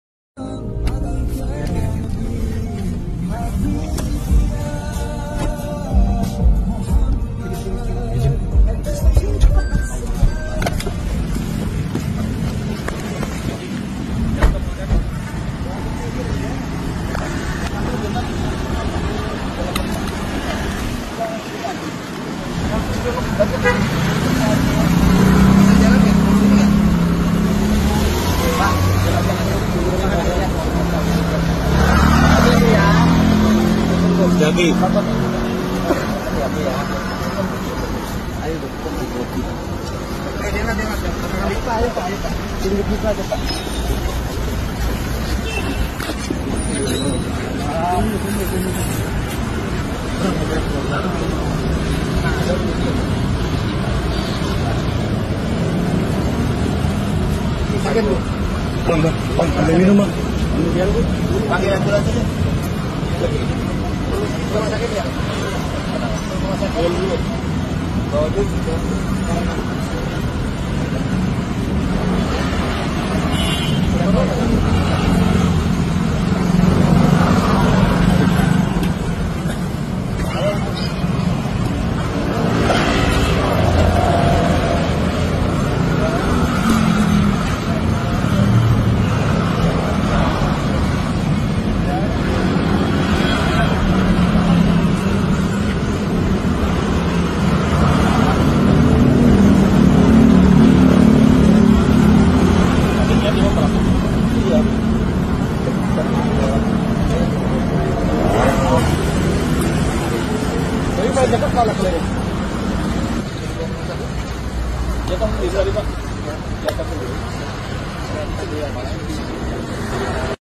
Kapolda Banten Tolong Korban Kecelakaan di Serang
Aksi Kapolda Banten, Irjen Pol. Suyudi Ario Seto yang langsung menolong korban kecelakaan lalu lintas di Kota Serang. Semoga apa yang dilakukan oleh Pak Kapolda ini bisa menjadi contoh yang baik bagi anggota polisi yang lain serta seluruh masyarakat Indonesia. Video : Ig polisi_indonesia